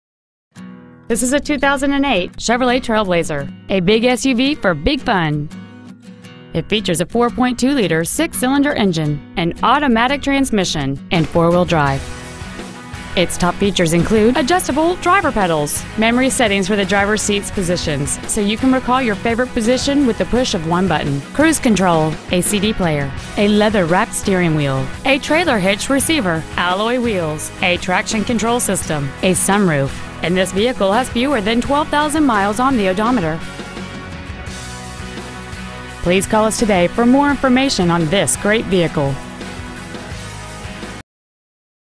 2008 Chevrolet TrailBlazer Puyallup WA
Year : 2008

 Make : Chevrolet

 Model : TrailBlazer

 Engine : 4.2L I6 MFI DOHC

 Trans . : Automatic

 Exterior : Red

 Miles : 11,070

 Interior : Black

 Sunset Chevrolet

 910 Traffic Ave

 Used 2008 Chevrolet TrailBlazer Sumner WA